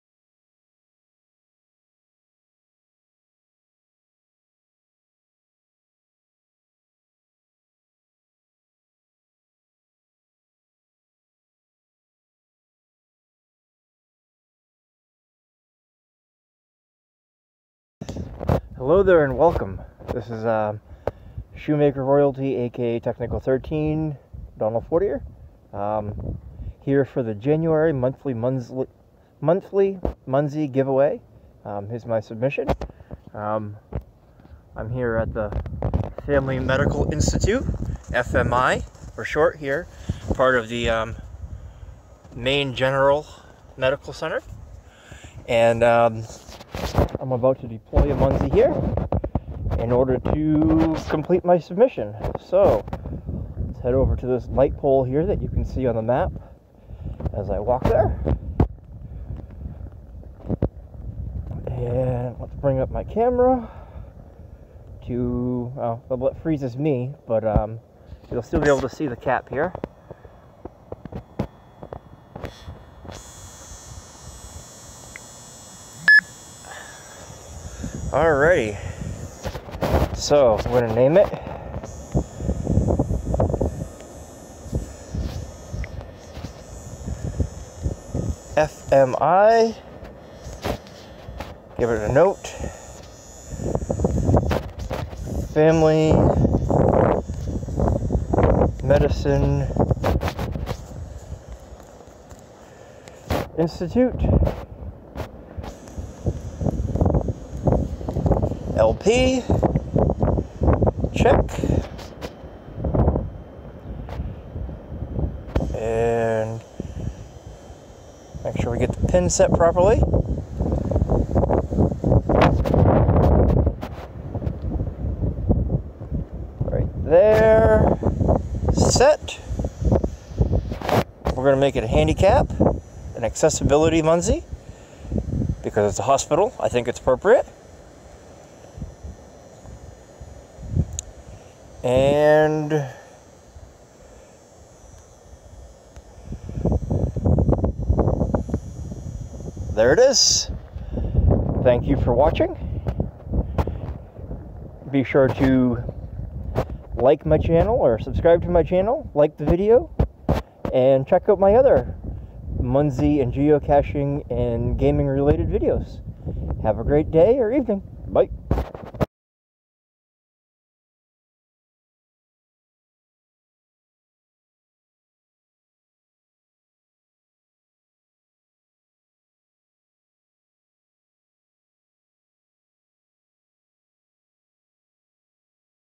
January 2018 Monthly Munzee entry
I deploy a Munzee at the local FMI hospital to complete the January 2018 MonthlyMunzeeGiveaway challenge!